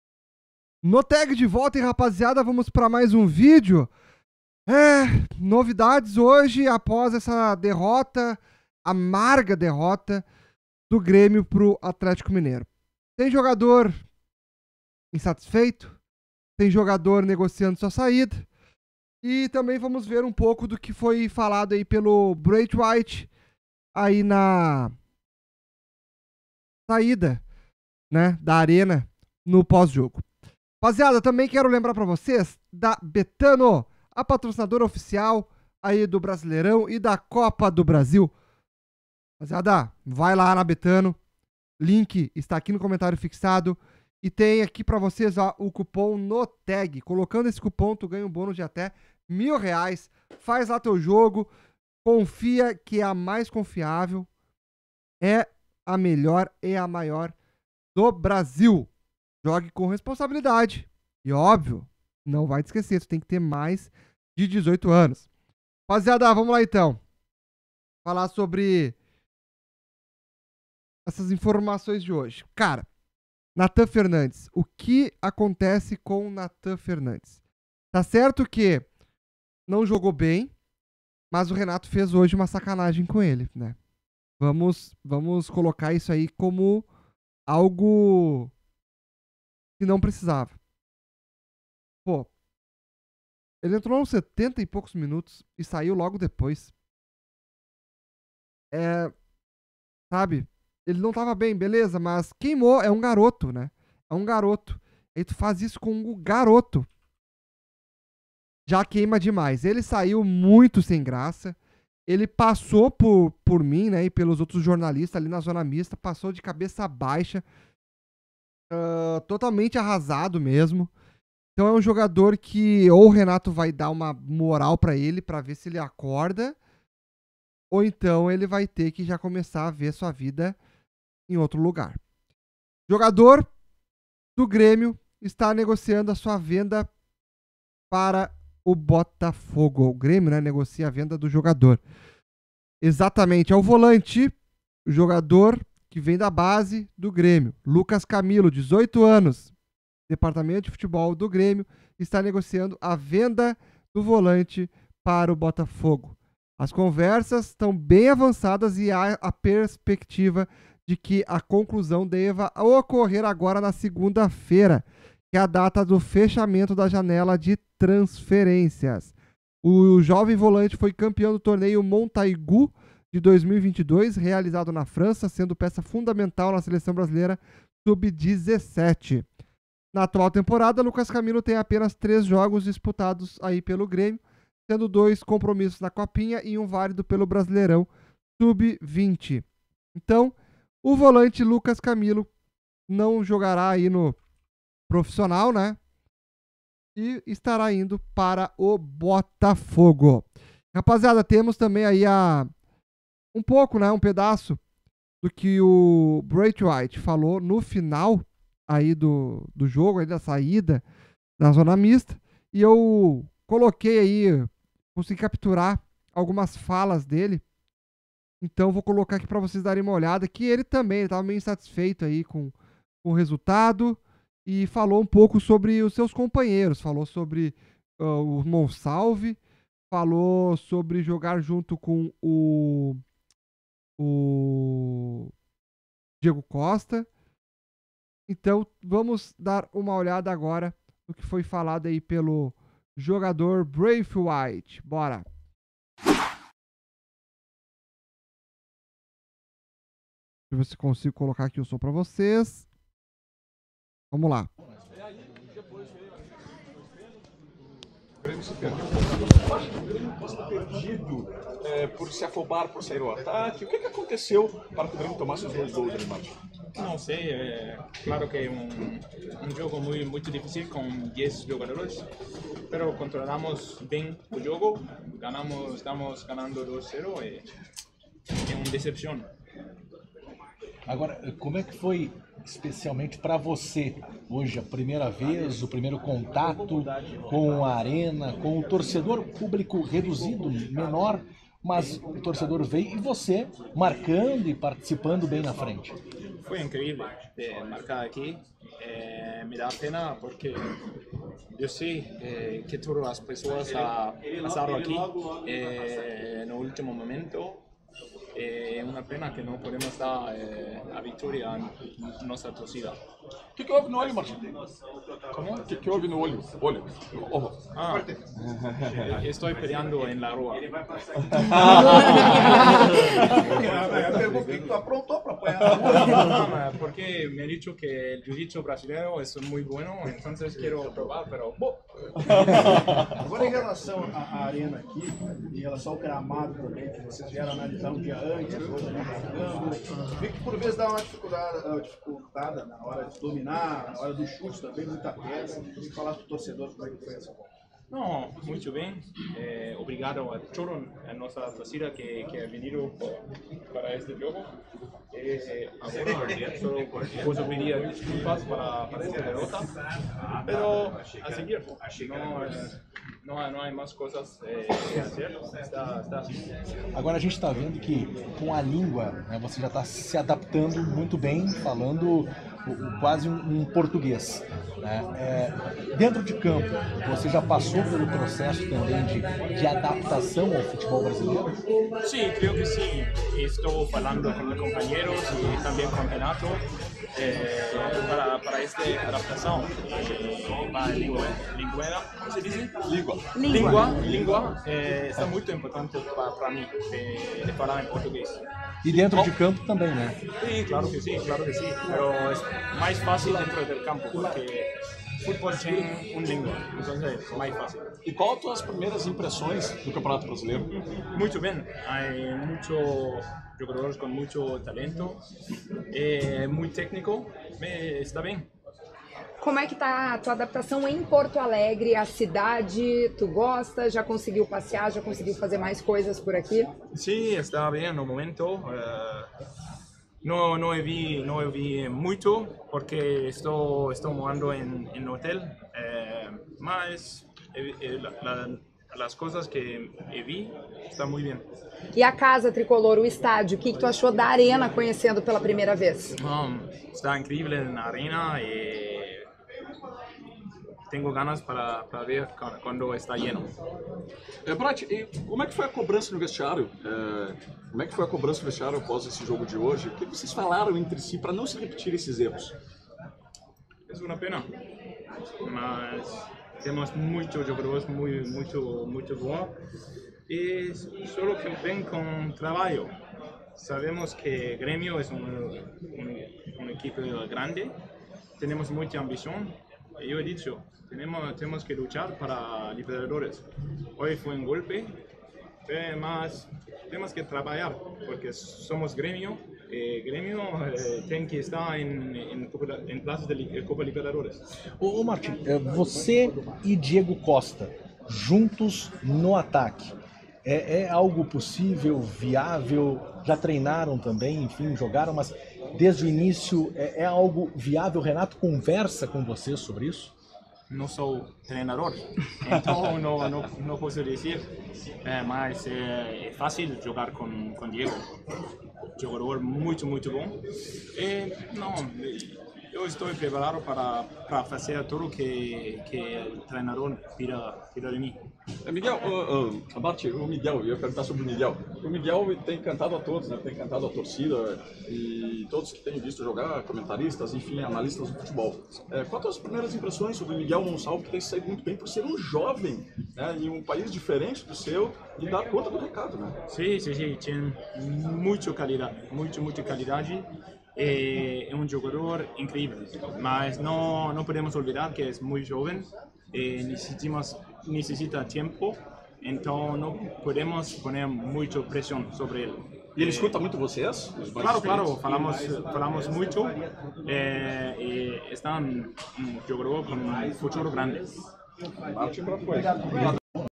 🚨JOGADOR INSATISFEITO, DE SAÍDA MAIS UM JOGADOR VENDIDO E BRAITHWAITE FALA SOBRE SITUAÇÃO DO GRÊMIO!
Jogador insatisfeito e mais jogador vendido!
Braithwaite ainda fala muito sobre a situação do imortal!